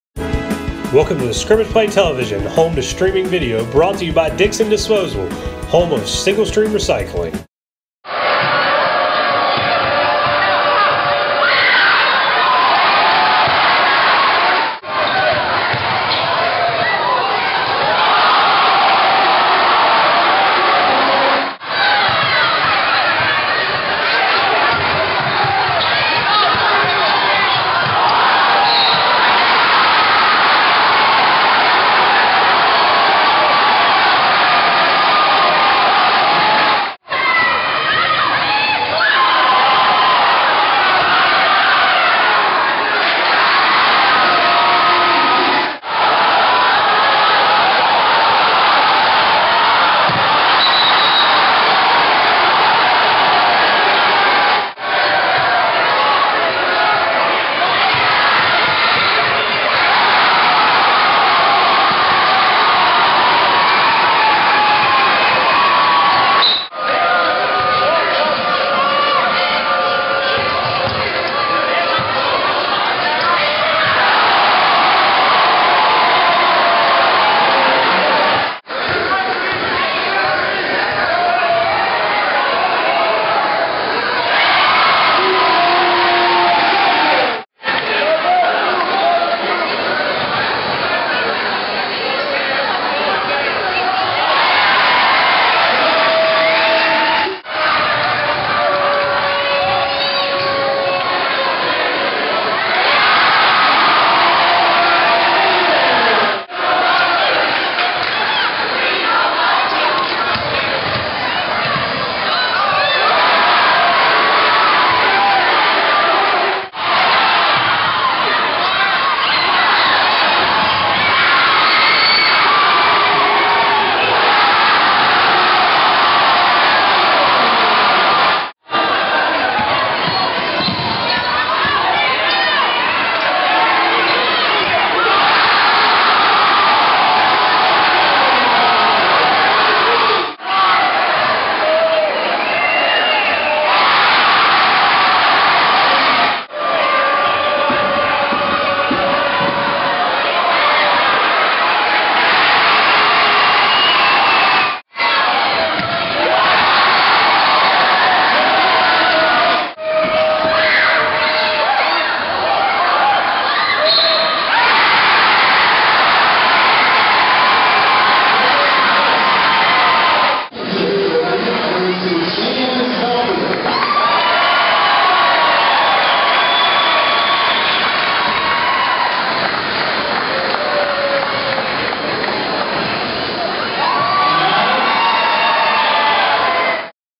STAB basketball beats Western 61-56
St. Anne's-Belfield picks up a victory over Western in the Holiday Classic final Monday night.